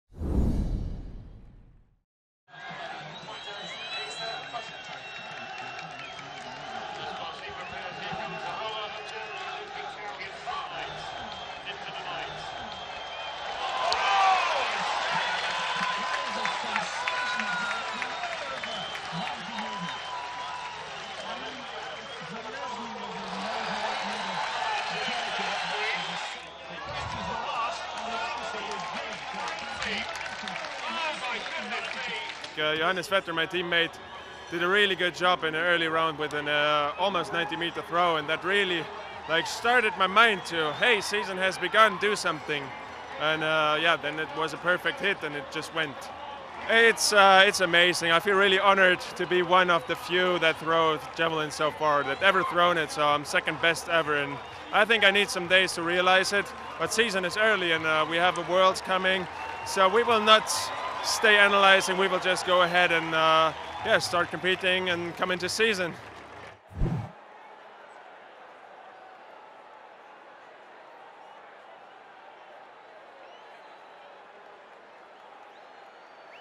Thomas Röhler unleashes a huge throw of 93.90 - Men's Javelin IAAF Diamond League Doha 2017
Thomas Röhler of Germany with the throw of 93.90m who puts him to the second of all time list.